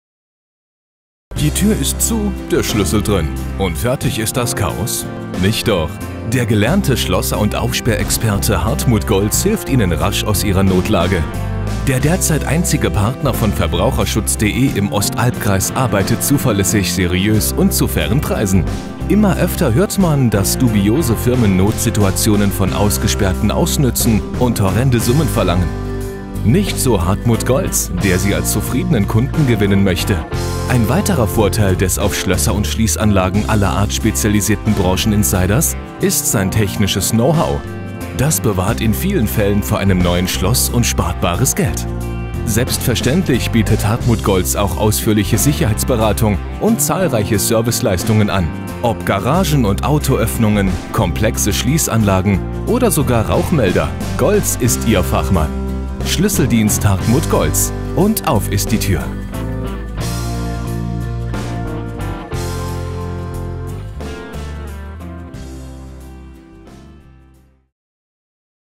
Schlüsseldienst Hartmut Golz in Ellwangen, Ostalbkreis - Sicherheitsberatung, Schlosserei
Die Tür ist zu, der Schlüssel drin — und fertig ist das Chaos? Nicht doch: Der gelernte Schlosser und Aufsperr-Experte Hartmut Golz hilft Ihnen rasch aus Ihrer Notlage!

Der derzeit einzige Partner von Verbraucherschutz.de im Ostalbkreis arbeitet zuverlässig, seriös und zu fairen Preisen. Immer öfter hört man, dass dubiose Firmen Notsituationen von Ausgesperrten ausnützen und horrende Summen verlangen — nicht so Hartmut Golz, der Sie als zufriedenen Kunden gewinnen möchte!
Ein weiterer Vorteil des auf Schlösser und Schließanlagen aller Art spezialisierten Branchen-Insiders ist sein technisches Know-how. Das bewahrt in vielen Fällen vor einem neuen Schloss und spart bares Geld.

Selbstverständlich bietet Hartmut Golz auch ausführliche Sicherheitsberatung und zahlreiche Serviceleistungen an: Ob Garagen- und Autoöffnungen, komplexe Schließanlagen oder sogar Rauchmelder – Golz ist Ihr Fachmann.

Schlüsseldienst Hartmut Golz — und auf ist die Tür!

Weitere Infos zum Schlüsseldienst Golz in Ellwangen im Ostalbkreis finden Sie

Zum Schlüsseldienst Hartmut Golz in Ellwangen/Röhlingen finden Sie ein ausführliches Porträt